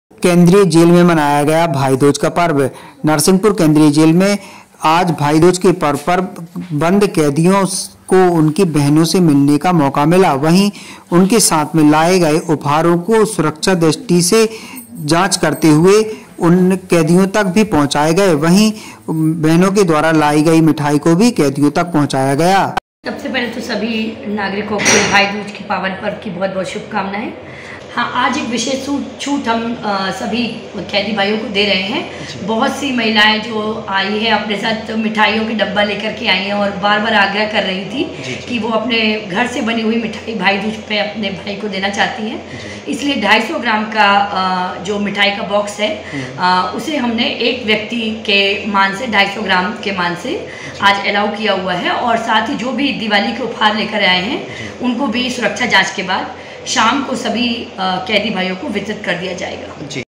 नरसिंहपुर : केंद्रीय जेल में मनाया गया भाईदूज का पर्व जेल में बंद बंदियों से मिली बहन
राजनीति,मनोरंजन,
बॉलीबुड, व्यापार और खेल में नवीनतम समाचारों को शामिल करता है